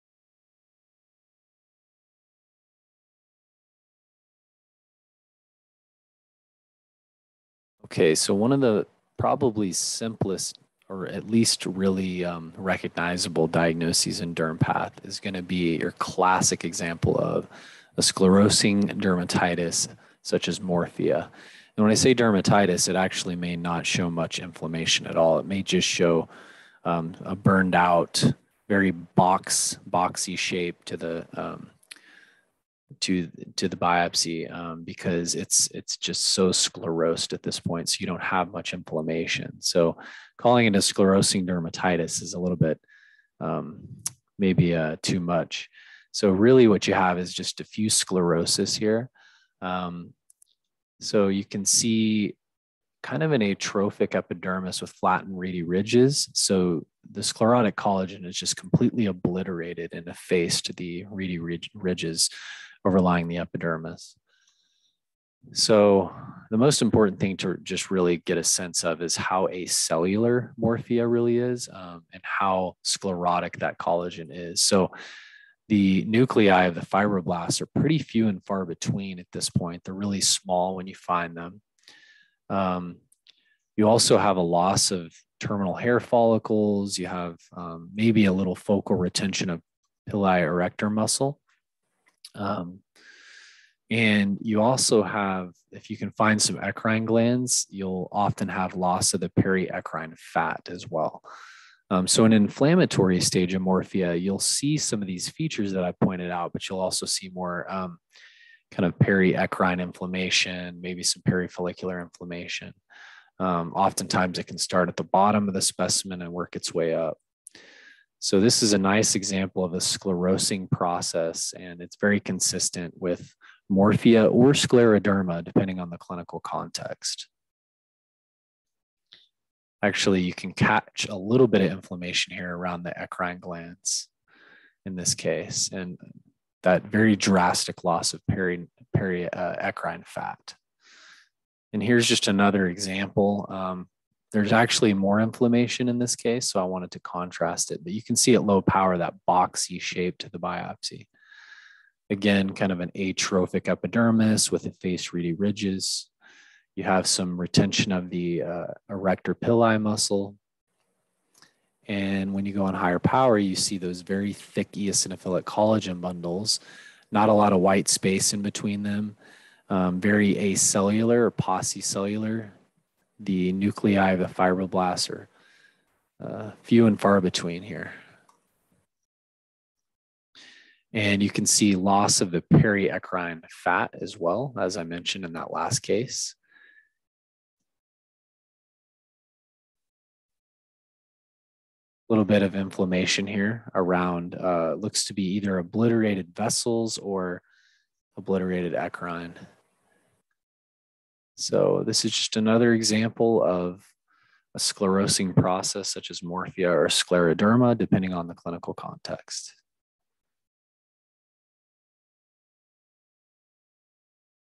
Morphea/scleroderma
Recognize dermal sclerosis and key features of morphea.